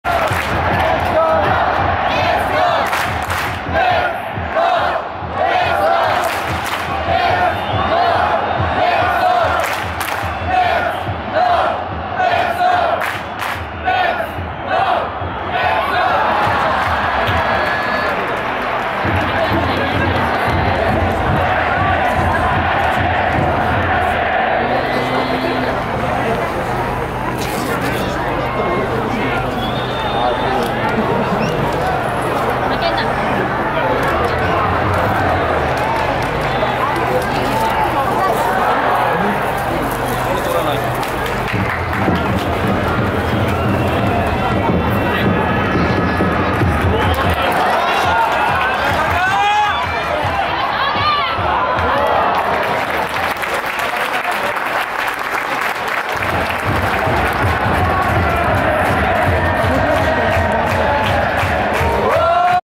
【Jリーグ】多摩川クラシコ！中村憲剛のCK vs FC東京の鉄壁守備！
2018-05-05
川崎フロンターレ vs FC東京
等々力競技場での多摩川クラシコ